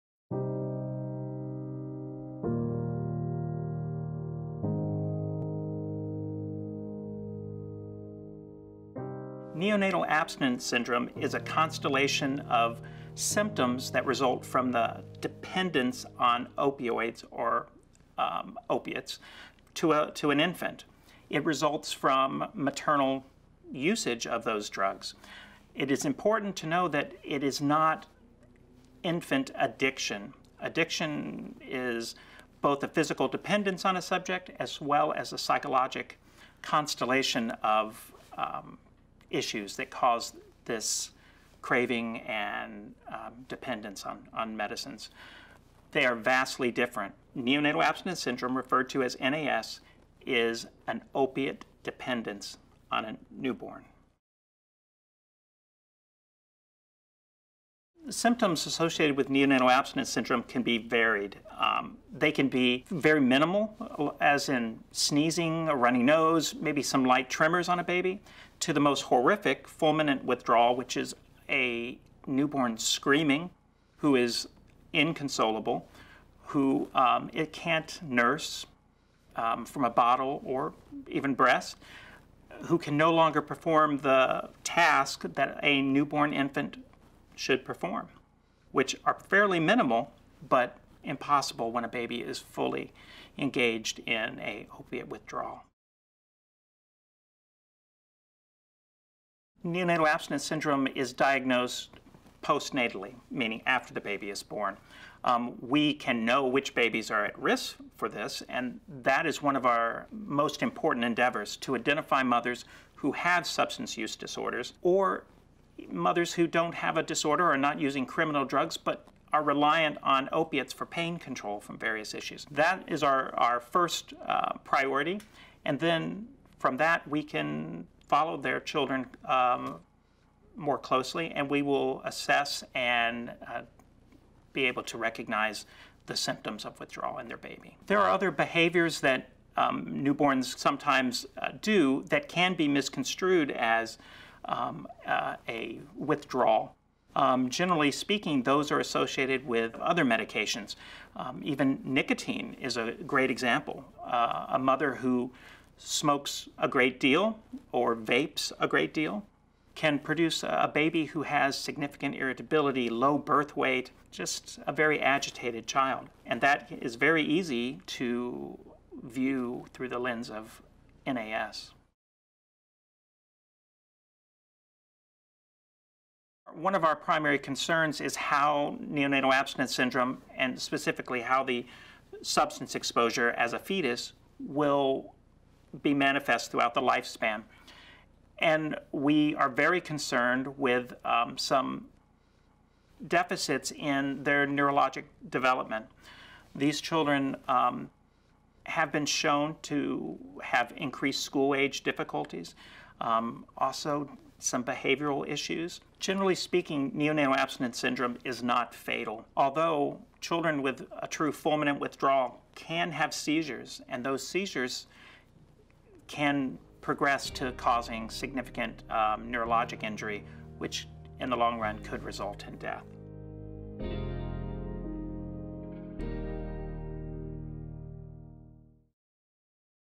Neonatal Abstinence Syndrome - Q & A with a neonatal nurse practitioner
Neonatal abstinence syndrome is found in babies born with substance dependence.  What are the symptoms, and what happens when a newborn withdraws?  How is it diagnosed and how does it affect the baby as he or she grows up?